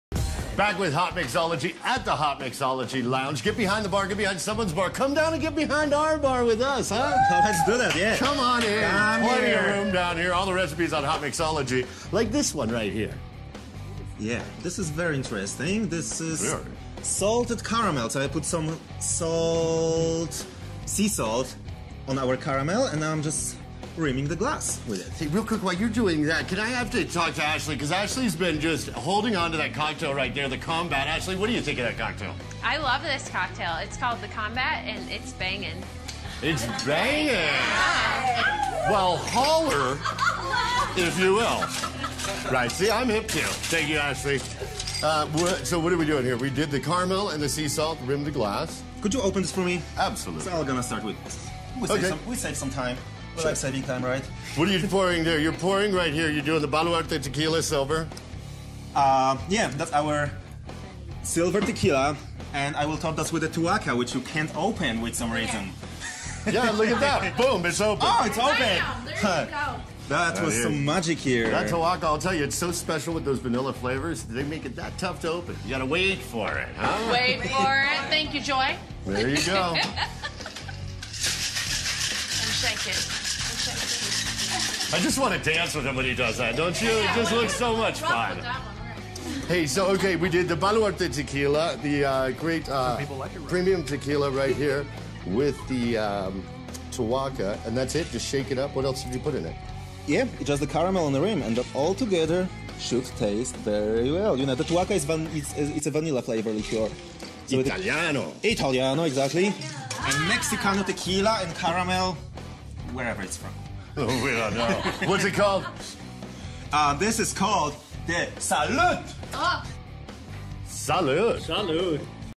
The Salute on Hotmixology
Tuaca and Baluarte Tequila makes a cocktail we call The Salute on Hotmixology